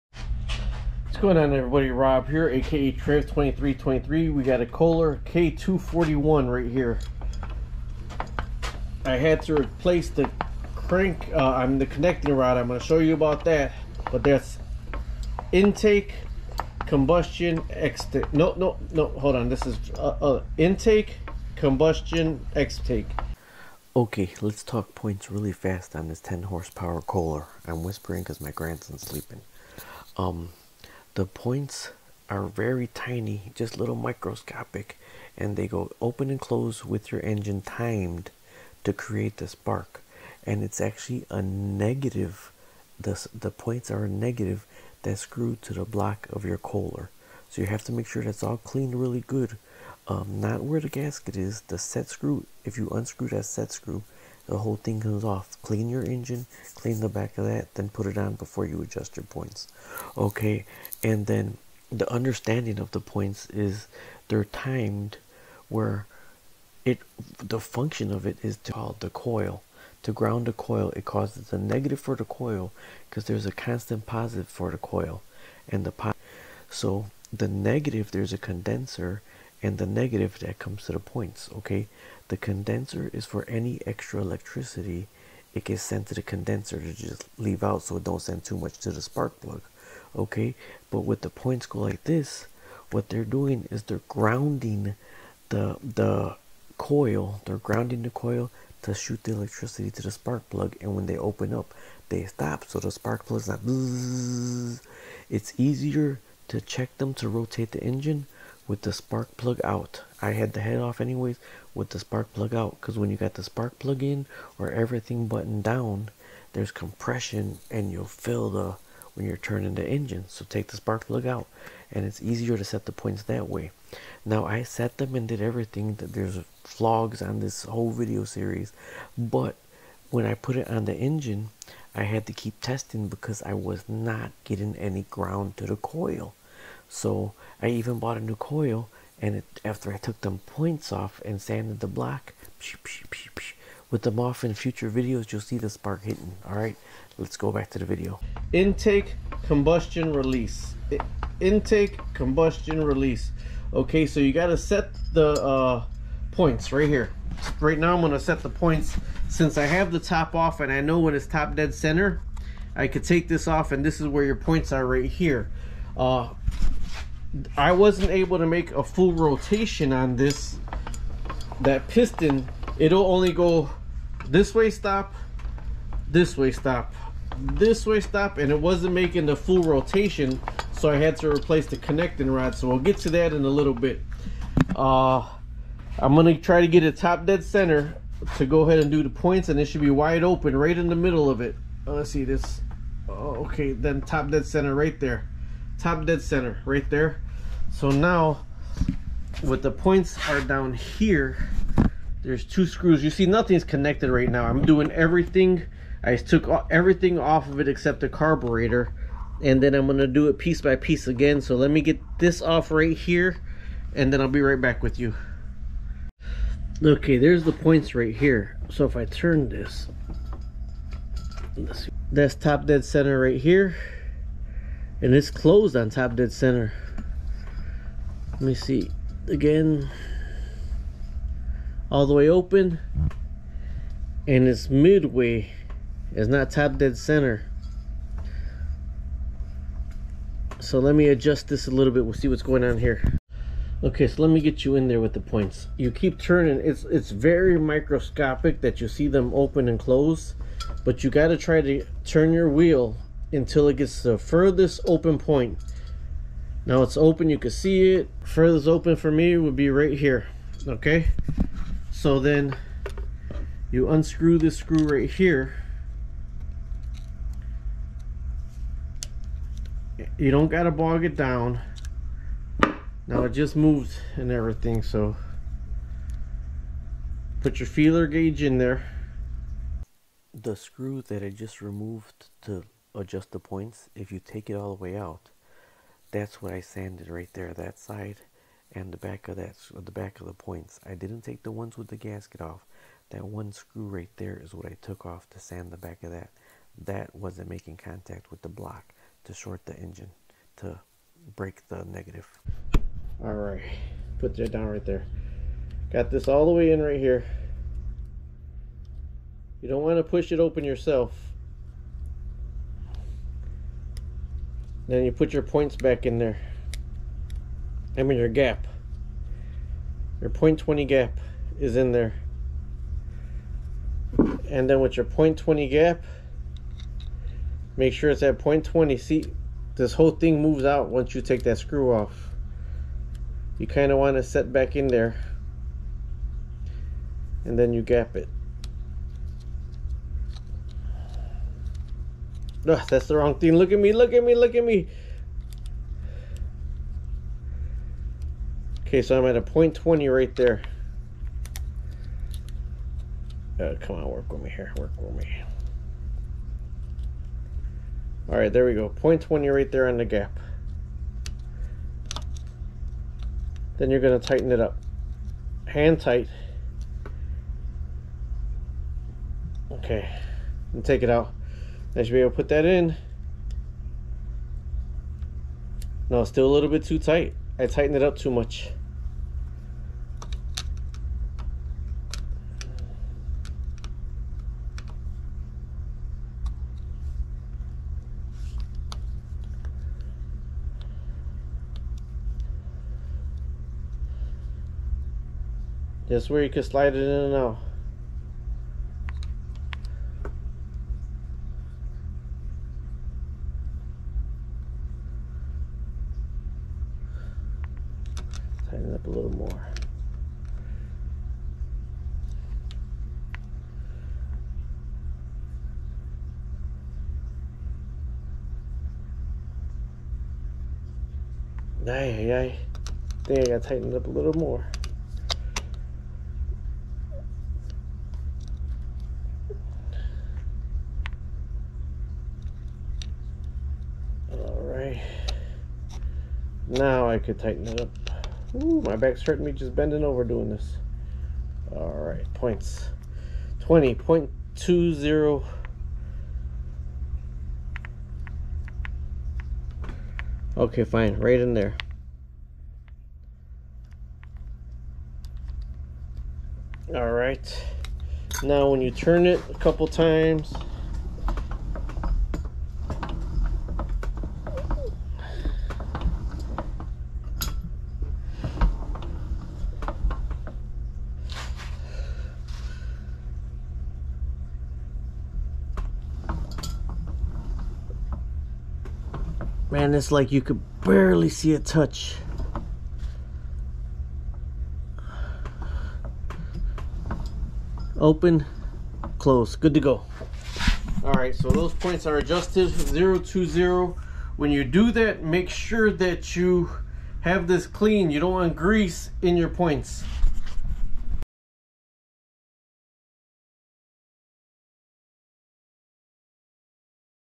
10 hp Kohler no spark Points explained
What's going on everybody Rob here with a little video on this 10 hp Kohler engine. I have a 1973 wheel horse tractor with a 10 hp Kohler engine. The Kohler engine was not getting any spark to the spark plug. Everything else was good even the points were adjusted. But when I found out after I had the engine on the tractor is the points were not cleaned as in the points were not grounding to the engine block. I had to remove the points the set screw on the points and took the points off of the engine to clean in saying that the engine block in the back side of the points in order to create the ground. That was the only thing causing the no spark on this 10 hp Kohler engine everything was right everything was proper. If you were to adjust the points or check the points you would see that they were proper at .020. Although troubleshooting and going back and forth I realized that the points were not grounding to the block. In this video I explained the points on the 1973 wheel horse tractor I explained what the procedure is and what the points are supposed to do on the 10 hp Kohler engine.

Rob here a.k.a. Trev2323